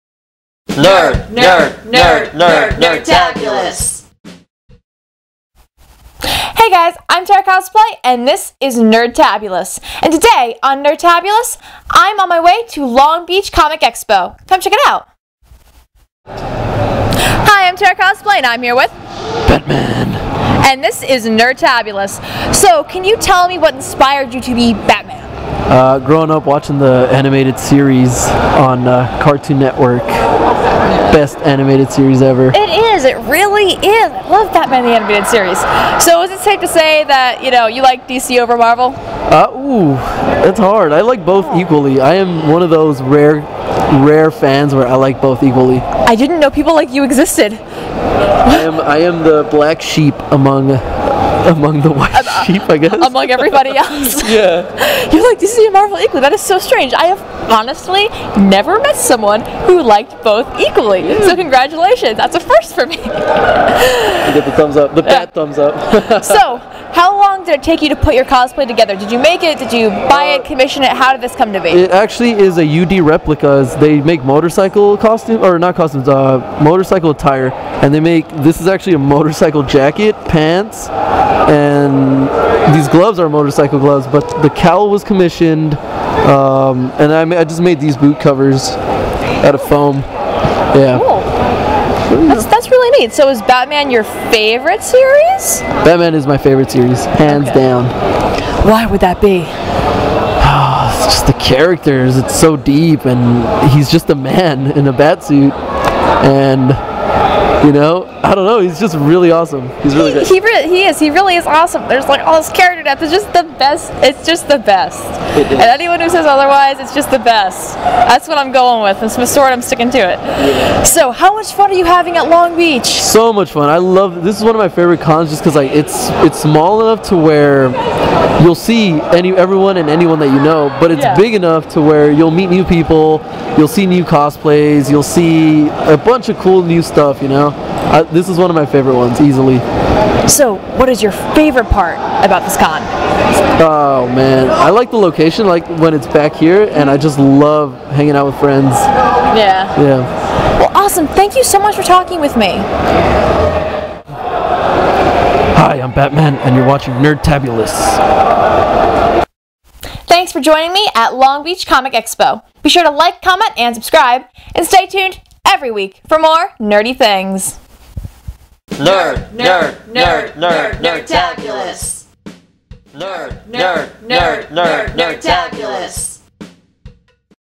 NERDTABULOUS - Long Beach Comic Expo 2017 - Part 6
Ep: 6.7 Tara Cosplay continues her journey at Long Beach Comic Expo and runs into Batman!!

Host
Tara Cosplay